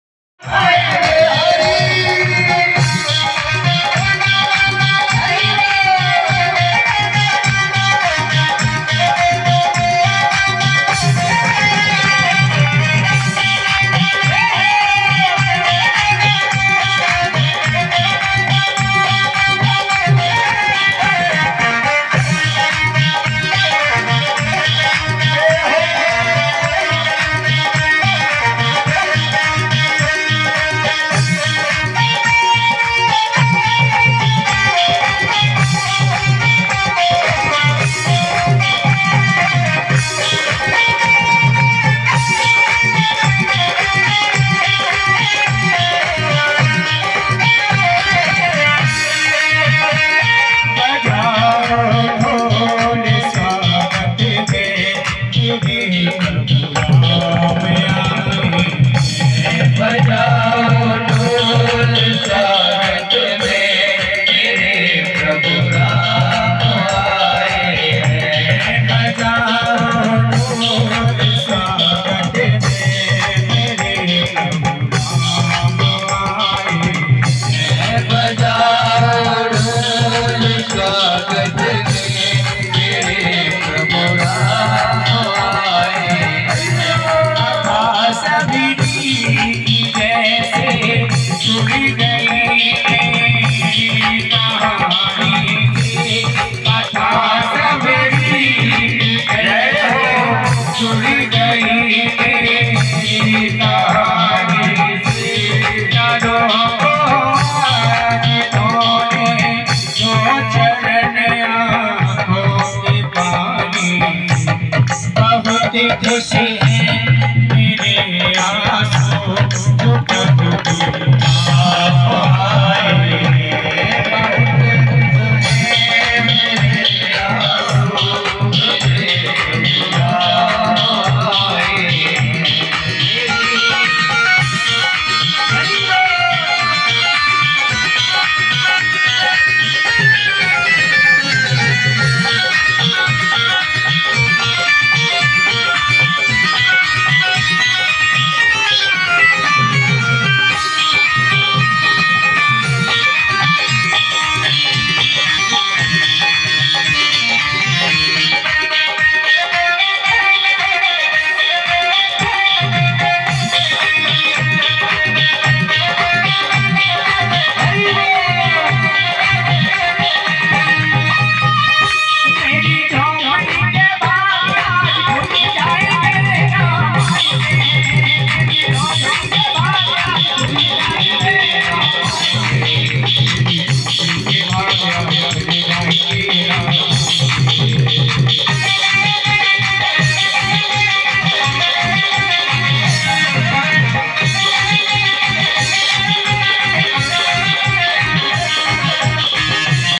Shri mad bhagwat katha
Shri mad bhagwat katha at jiwajipur darwar Vidisha Madhya Pradesh